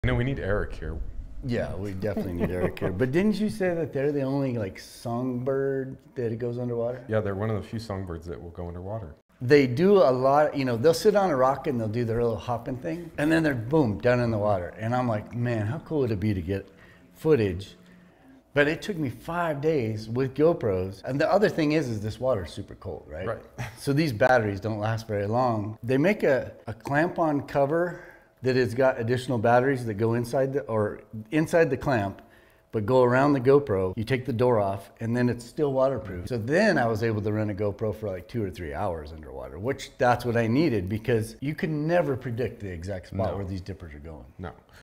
Amazing Underwater Footage of Songbirds Rare Sights and Chilly Adventures
Join Michael and Brandon on a fascinating journey with Truth and Legend Productions as we uncover the secrets of one of nature's marvels - the American Dipper, an aquatic songbird. After days of perseverance with GoPros in chilly waters, we've captured something truly remarkable. Dive in with us to witness the extraordinary capabilities of this unique bird.